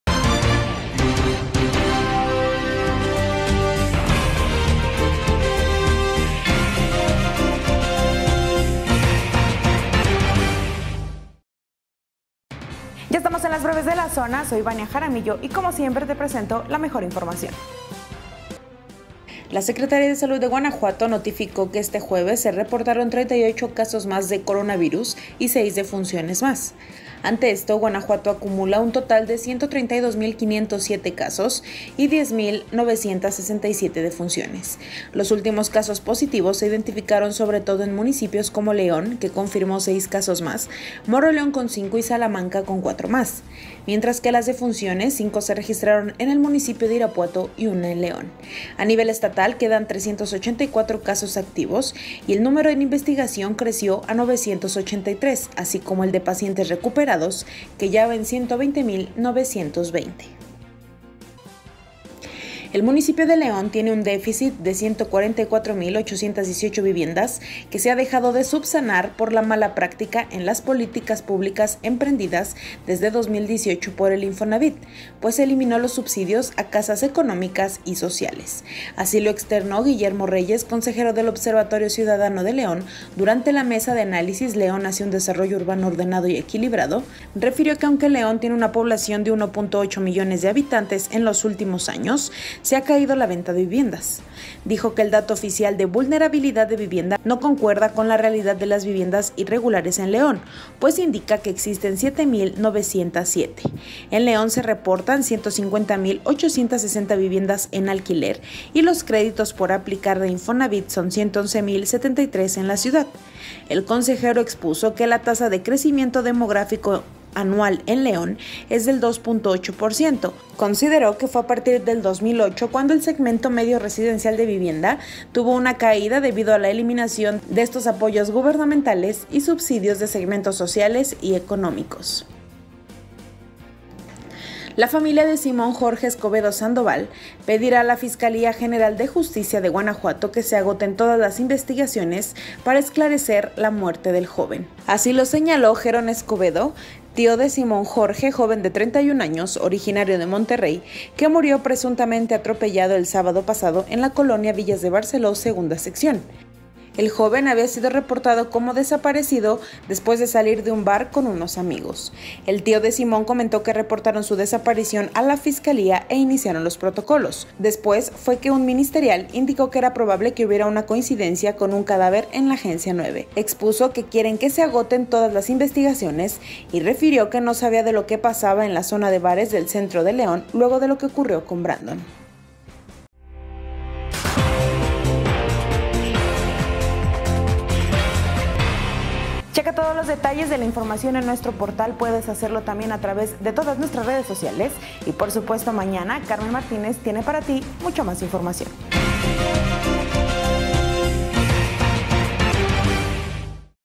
En las últimas horas, 6 muertes y 38 casos más de COVID-19 | BrevesDeLaZona📰
Te dejamos las ÚltimasNoticias de hoy
- Eliminación de subsidios ‘estancó’ el desarrollo de viviendas con Infonavit: OCL.
- Agotar todas las líneas de investigación, pide familia de Simón, joven fallecido en Villas de Barceló.
- En las últimas horas, seis defunciones y 38 casos más de COVID-19.